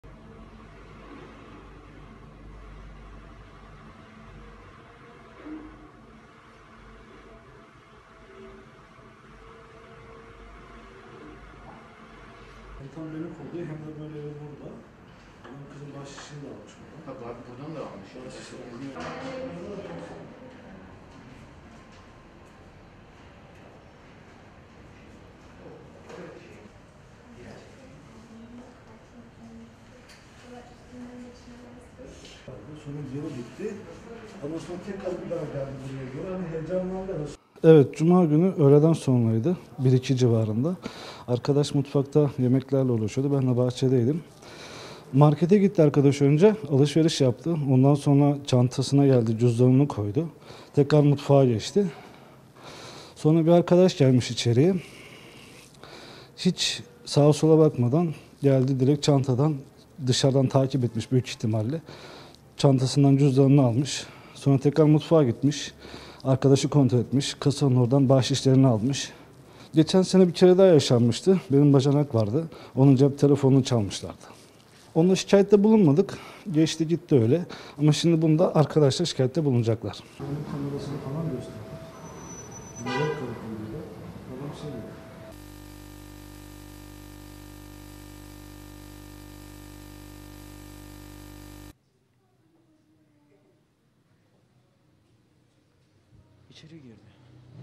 Gündüz Vakti Restorandan Cüzdan Çalan Hırsızın Rahat Tavırları Pes Dedirtti
Şişli’de bir restoranda çalışan kadının çantası kendisini takip eden hırsız tarafından çalındı. Hırsızın çantanın içindeki cüzdanı eliyle koymuş gibi bulup çaldığı anlar saniye saniye güvenlik kameralarına yansıdı.
Olay, geçtiğimiz Cuma günü öğle saatlerinde Şişli’de bulunan Kore yemekleri sunulan bir restoranında meydana geldi. Edinilen bilgiye göre, restoranda çalışan kadın alışveriş için yan tarafta bulunan markete gitti. Alışverişinin tamamlamasının ardından çalıştığı restorana geri dönen kadın içinde cüzdanı bulunan çantasını masanın üzerine koydu ve mutfağa girdi. Kadın bir süre sonra çantasını bıraktığı yerden aldı ve iddiaya göre içini açtığında cüzdanın yerinde olmadığını fark etti. Soyulduğunu anlayan kadın durumu restoran sahibine anlattı.
Güvenlik kameraları izlenince gerçek ortaya çıktı
Restoran sahibi Muhammed Ali Tüfekçi ve kadın çalışan ne olup bittiğini anlamak için mekanın güvenlik kamerası kayıtlarını izleyince gerçek ortaya çıktı. Güvenlik kamerası kayıtlarını incelediklerinde kadından bir süre sonra içeri giren bir kişinin kasanın yanında bulunan paraları ve kadının çantasının içindeki cüzdanı çaldığını gördüler.
“Büyük ihtimalle dışarıda takip etmiş”
Kayıtları izledikten sonra şoke olduklarını söyleyen restoran sahibi Muhammed Ali Tüfekçi hala şaşkın olduklarını ifade ederek, “Cuma günü öğleden sonra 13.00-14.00 civarıydı. Arkadaş mutfakta yemeklerle uğraşıyordu, ben de bahçedeydim. Arkadaş markete gidip alışveriş yaptı. Buraya gelip çantasını masanın üzerine koydu ve tekrar mutfağa geçti. Sonra içeri bir arkadaş geldi ve hiç sağa sola bakmadan çantadan cüzdanı aldı. Büyük ihtimalle dışarıda takip etmiş. Daha sonra tekrar mutfağın yanına gidip arkadaşı kontrol etti ve kasanın yanındaki bahşişleri aldı. Daha önce geçen yıl burada benim bacanağın cep telefonu çalınmıştı. O olayda şikayette bulunmamıştık ama şimdi arkadaş şikayette bulunacak” ifadelerini kullandı.
Rahat tavırları pes dedirtti
Öte yandan güvenlik kameralarına yansıyan görüntülerde şüphelinin içeri girdiği, önce etrafı kolaçan ettiği daha sonra kasanın yanındaki paraları alıp cebine koyduğu görüldü. Bunun yanı sıra şüphelinin masanın üzerinde duran çantanın yanına gittiği ve çantayı açtığı, cüzdanı aldığı, daha sonra ise oradan ayrıldığı güvenlik kameraları tarafından saniye saniye kaydedildi.